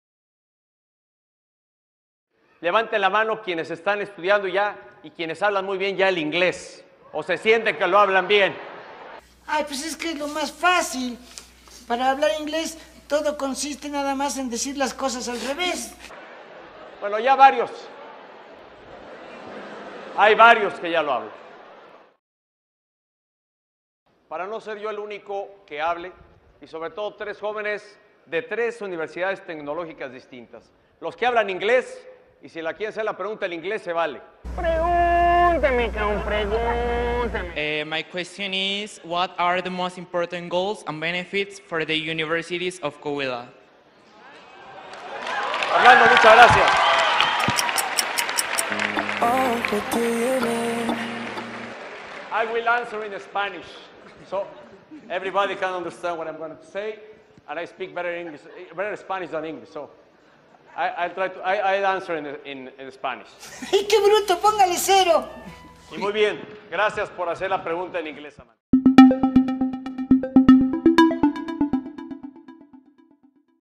Así está el nivel de inglés de Enrique Peña Nieto
CON CARA DE WHAT? Aquí puedes ver cómo anda el NIVEL DE INGLÉS de Enrique Peña Nieto, tras un evento con universitarios en Coahuila... COMPARTE si te gustaría que te ayudara a ser bilingüe...